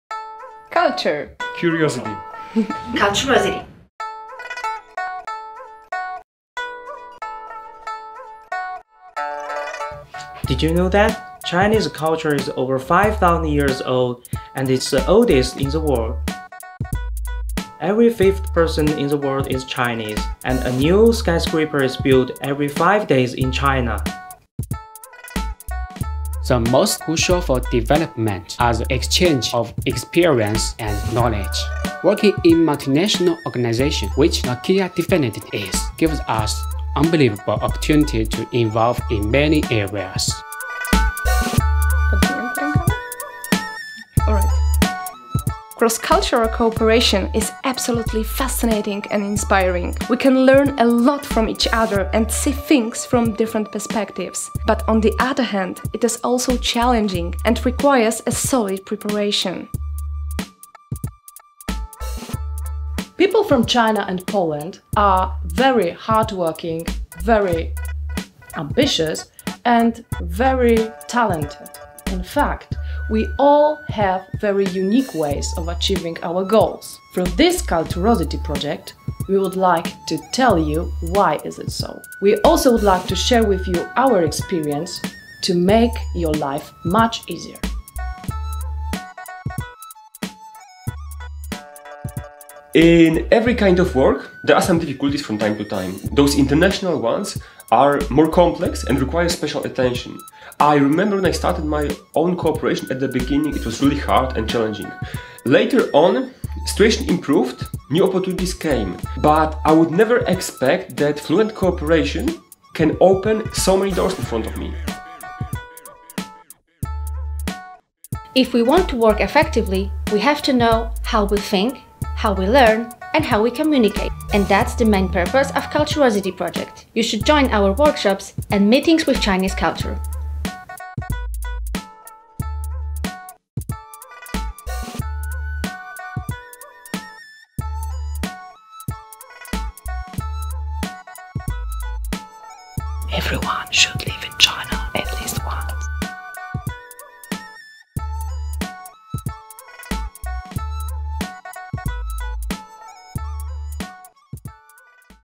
NOKIA Culturosity
Pracując w Nokii na co dzień spotykamy się z przedstawicielami wielu kultur.
Jest to niesamowicie inspirujące i rozwijające doświadczenie, niemniej jednak wymaga solidnego przygotowania.
Właśnie w tym celu powstał program Culturosity – aby pomóc ludziom z różnych kręgów kulturowych efektywnie się porozumiewać,
tworzyć zgrany zespół, budować solidne relacje zawodowe i osobiste.

Zaczęliśmy od współpracy z kolegami z Chin. Prowadzimy szkolenia i eventy, których celem jest lepsze zrozumienie mieszkańców Państwa Środka oraz poprawa jakości i efektywności wspólnie prowadzonych projektów. Mamy za sobą pierwsze radości i sukcesy, ale największą satysfakcją są pozytywne opinie uczestników treningów.